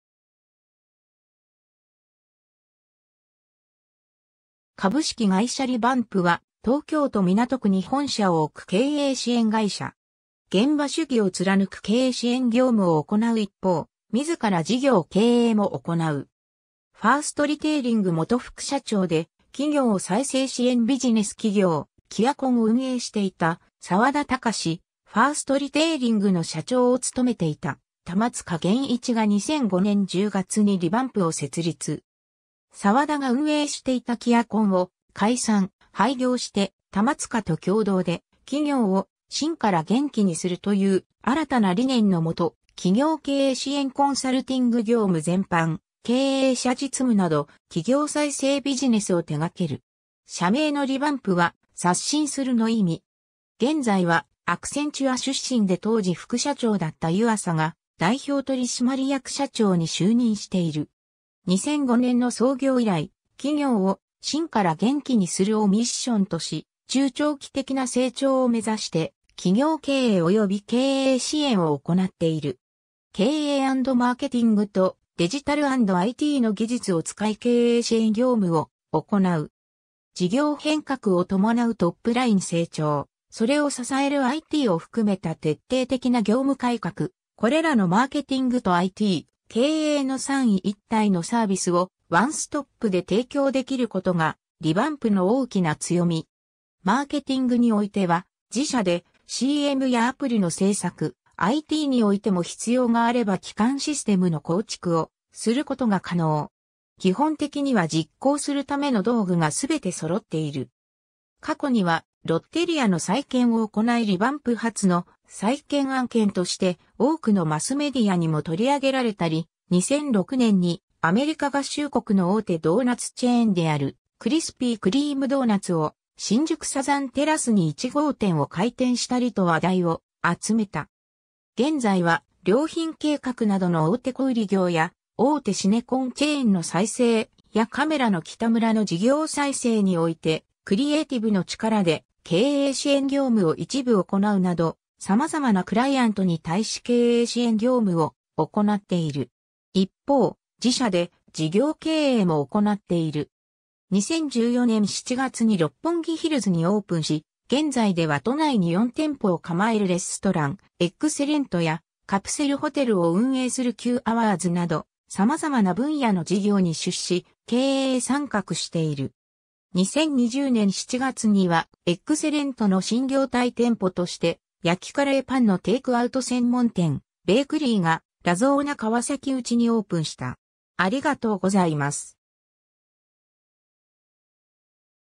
リヴァンプ
株式会社リヴァンプ (Revamp Corporation) は、東京都港区に本社を置く経営支援会社。
現場主義を貫く経営支援業務（コンサルティング）を行う一方、自ら事業経営も行う。
 ファーストリテイリング元副社長で企業再生支援ビジネス企業キアコンを運営していた澤田貴司、ファーストリテイリングの社長を務めていた玉塚元一が2005年10月にリヴァンプを設立。
 澤田が運営していたキアコンを解散・廃業して、玉塚と共同で"企業を芯から元気にする"という新たな理念の元、企業経営支援コンサルティング業務全般、経営者実務など企業再生ビジネスを手掛ける。
社名のリヴァンプは「刷新する」の意味。
 現在は、アクセンチュア出身で当時副社長だった湯浅が代表取締役社長に就任している。
 2005年の創業以来、“企業を芯から元気にする”をミッションとし、中長期的な成長を目指して企業経営および経営支援を行っている。
「経営＆マーケティング」と「デジタル＆IT」の技術を使い経営支援業務を行う。
事業変革を伴うトップライン成長、それを支えるITを含めた徹底的な業務改革、これらのマーケティングとIT、経営の三位一体のサービスをワンストップで提供できることがリヴァンプの大きな強み。
マーケティングにおいては自社でCM やアプリの制作、ITにおいても必要があれば基幹システムの構築をすることが可能。
基本的には実行するための道具がすべて揃っている。
 過去には、ロッテリアの再建を行いリヴァンプ初の再建案件として多くのマスメディアにも取り上げられたり、 2006年にアメリカ合衆国の大手ドーナツチェーンであるクリスピー・クリーム・ドーナツを、新宿サザンテラスに1号店を開店したりと話題を集めた。
 現在は、良品計画などの大手小売業や、大手シネコン・チェーンの再生、やカメラのキタムラの事業再生においてクリエイティブの力で経営支援業務を一部行うなど、様々なクライアント に対し経営支援業務を行っている。
 一方、自社で事業経営も行っている。
2014年7月に六本木ヒルズにオープンし現在では都内に4店舗を構えるレストラン「eggcellent（エッグセレント）」や、カプセルホテルを運営する9 hoursなど、様々な分野の事業に出資、経営参画している。
 2020年7月には、eggcellentの新業態店舗として、焼きカレーパンのテイクアウト専門店「ベイクリー（BAKURRY）」がラゾーナ川崎内にオープンした。